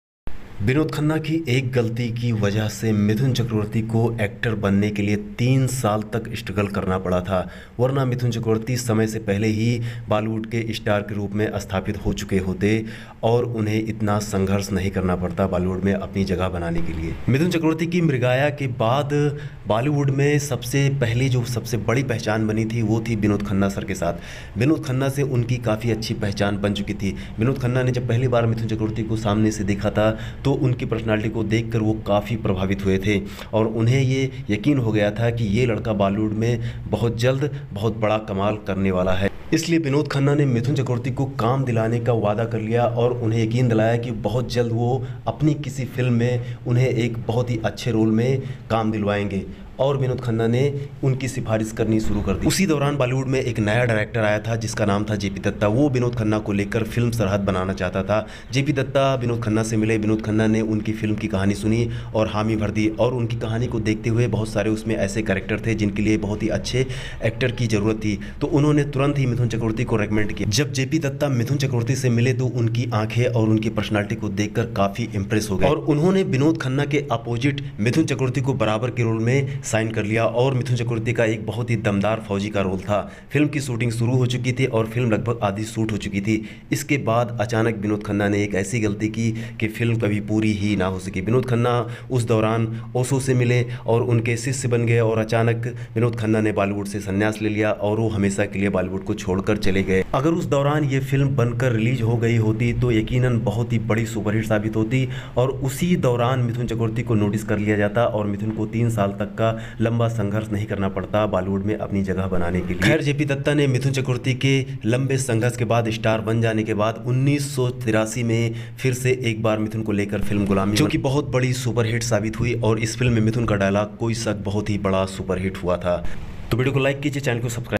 Mithun chakraborty को Vinod khanna की एक गलती की वजह से 3 साल struggle करना पड़ा था
Mithun chakraborty ko vinod khanna ki ek galti ki wajah se 3sal struggle karna pada tha

Vinod khanna, Vinod khanna songs, Vinod khanna movies